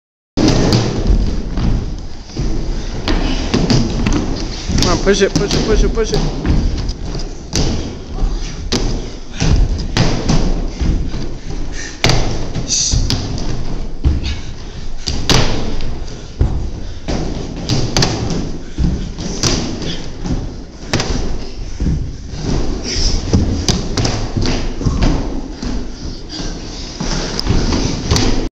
Fillmore Boxing Club: Exercises
Fillmore Boxing Club:  The Youth Group Class exercises to improve the muscle endurance of their legs, arms, and chest. (2 minute drill)